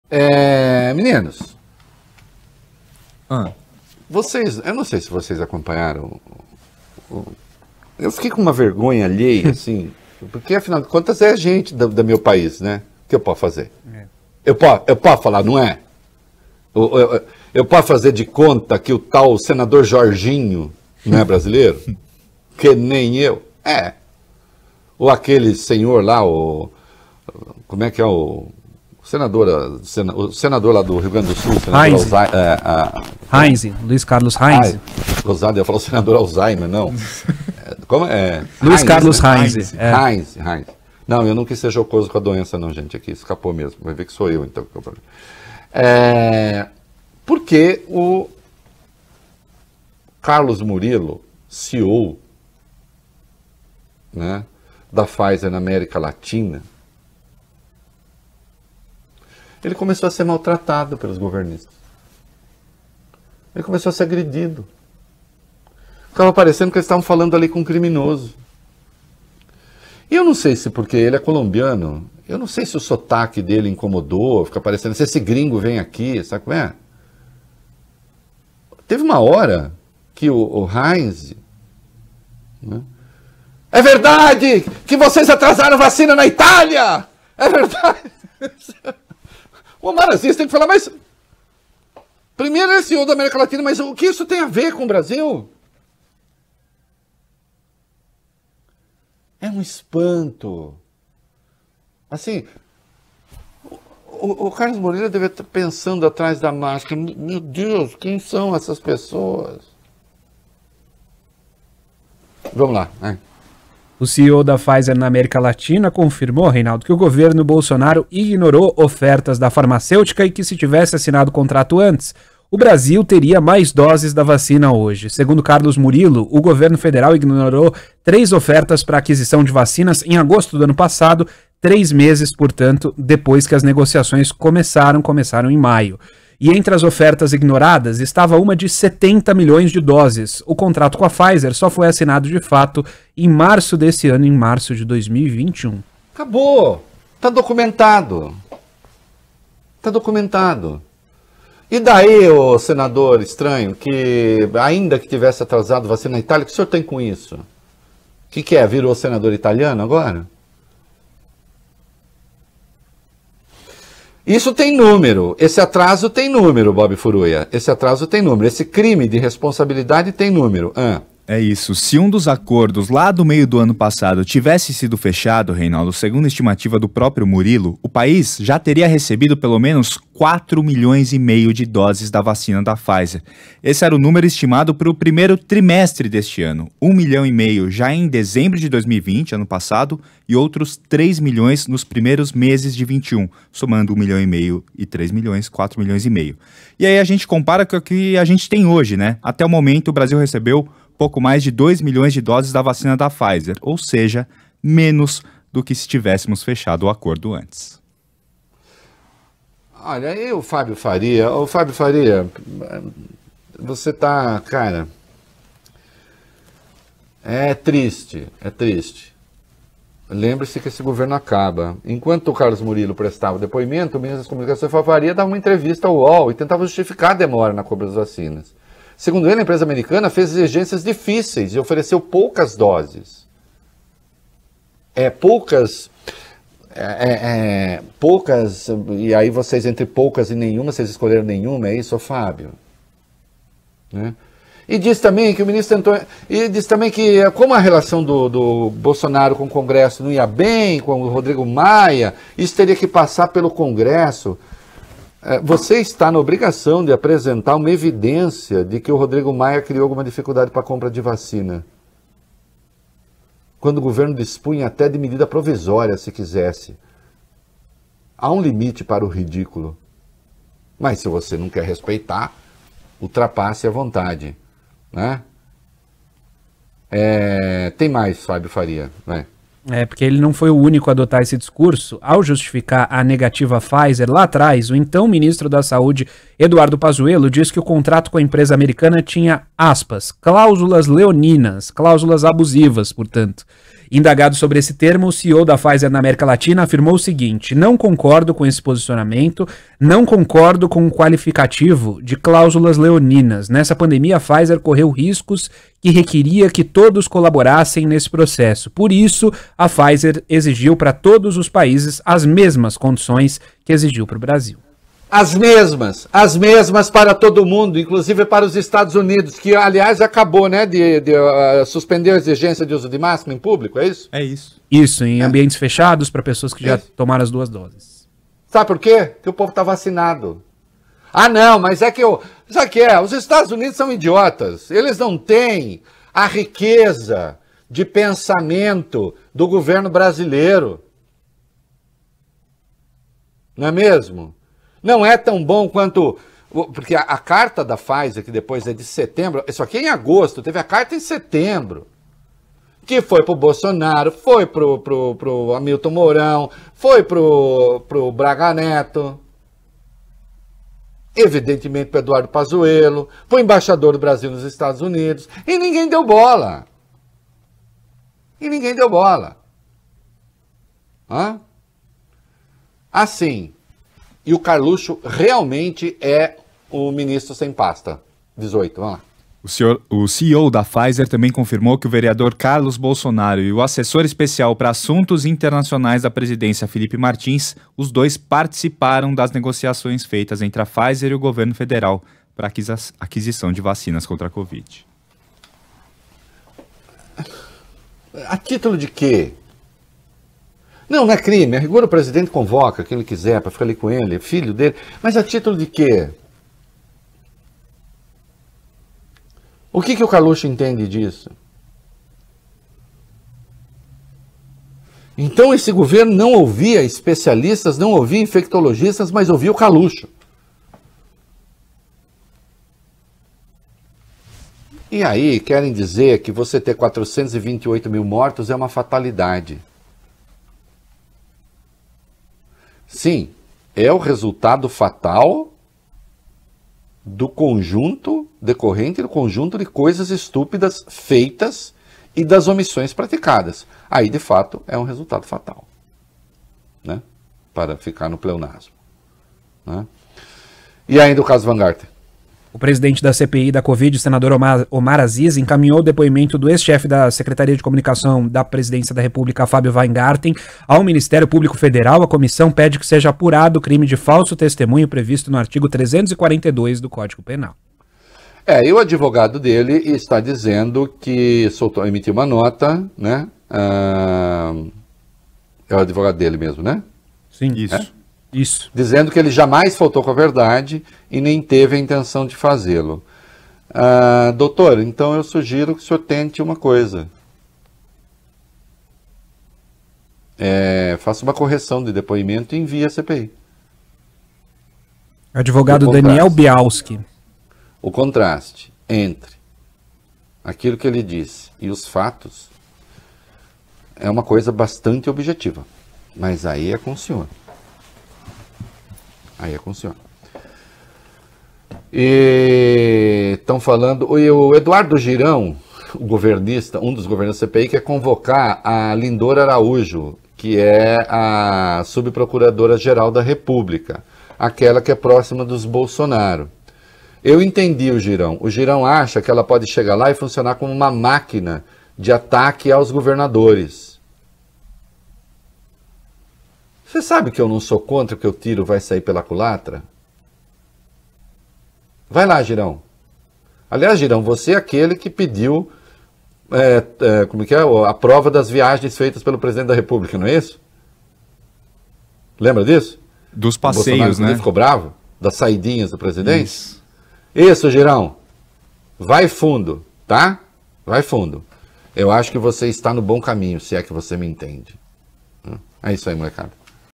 Reinaldo Azevedo: Depoimento de diretor da Pfizer foi espetáculo de vergonha alheia. Dos governistas
Reinaldo Azevedo comenta o depoimento do CEO da Pfizer na América Latina para a CPI da Covid-19. O âncora da BandNews FM fala sobre o tratamento dado a Carlos Murillo pelos senadores governistas. O jornalista ainda lembra que o governo recusou vacinas da Pfizer em diferentes oportunidades.